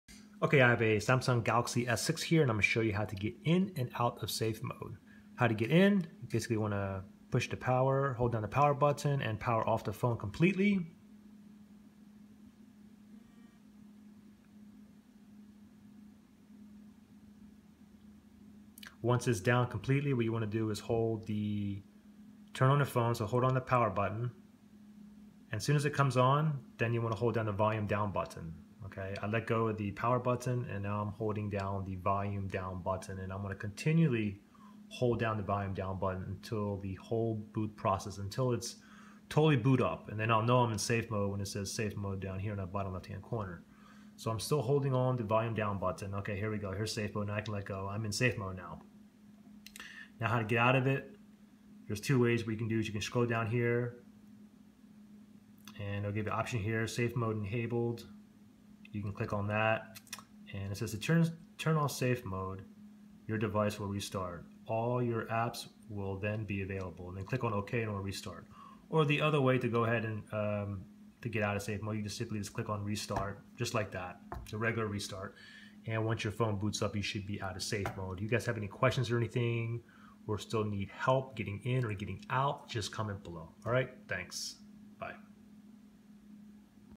Galaxy S6 / Edge: How to Enter & Exit SafeMode "Safe Mode"
In this video I show you how to get in and get out of safemode on your Samsung Galaxy S6 / Edge.
Safe Mode loads your phone in a diagnostic state (only preloaded apps and basic drivers are loaded, NO third party apps are loaded!) so you can determine if a third-party app is causing your device to freeze, lag, or malfunction other apps and functions. In other words if your phone was lagging, freezing or another app was malfunctioning or causing errors and then you have no issues when in safemode then you know its one or more of the  third party app that you downloaded that is causing this. So you need to uninstall and/or re-download that particular third party app.

Appreciate any Subs. Thank you.